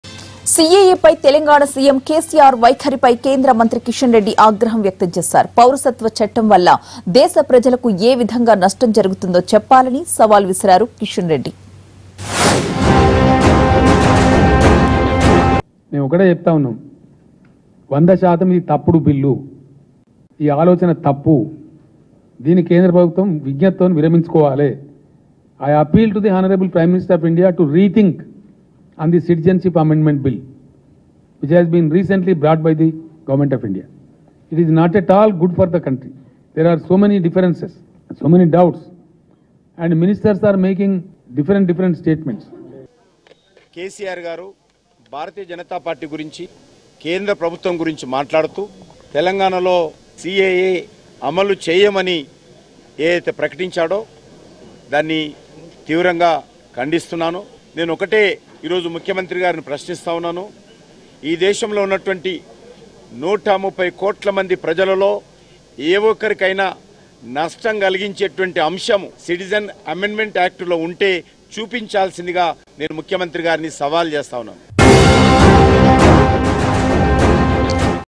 Kishan Reddy Straight Question To CM KCR Over CAA and NRC Bill | NTV
Kishan Reddy Straight Question To CM KCR Over CAA and NRC Bill